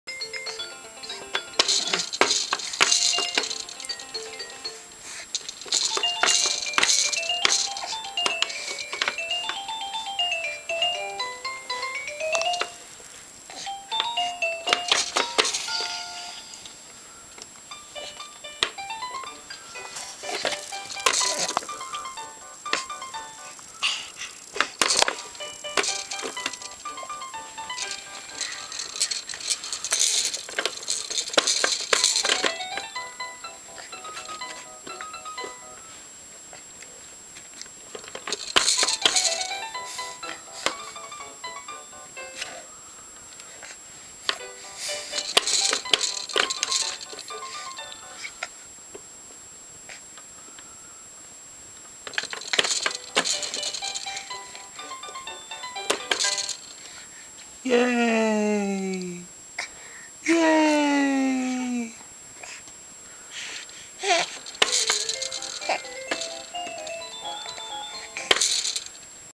Norah Hope...Live From Her Crib!!
Norah plays piano.  Practice...practice...practice!